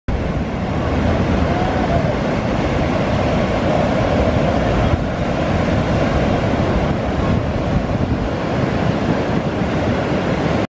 Ocean City, NJ at Sunset
Shot some video on a September evening, as the sun goes down.